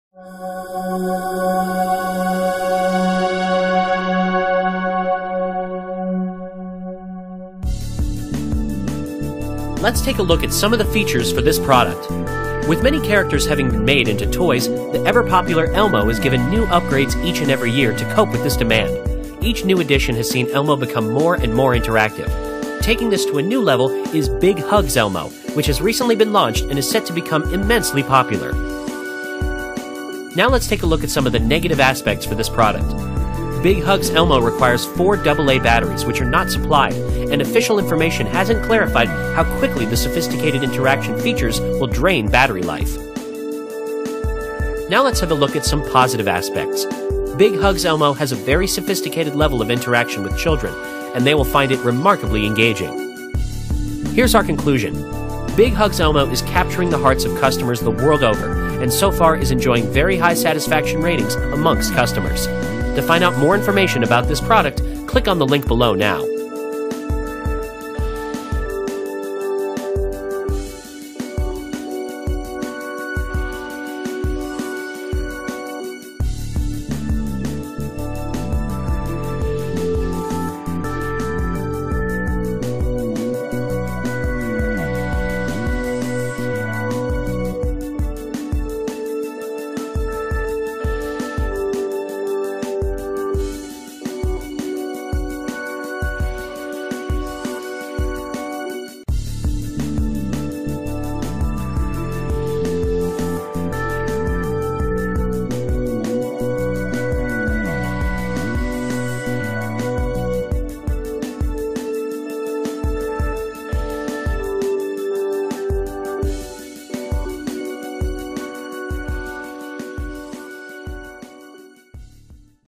Christmas Toys For Kids | Big Hugs Elmo Review
Big Hugs Elmo Review

Big Hugs Elmo is 22" tall and made of delightfully cuddly soft plush fabric and he responds to hugs and cuddles. He is interactive enough to feel almost like the "real thing". He sings several songs, including a lullaby when he's being rocked to sleep, can actively encourage the child's imagination with different suggestions of pretend play. He can interact when he's being rocked or bounced around and is even sensitive enough to tell the child when he's being held upside down.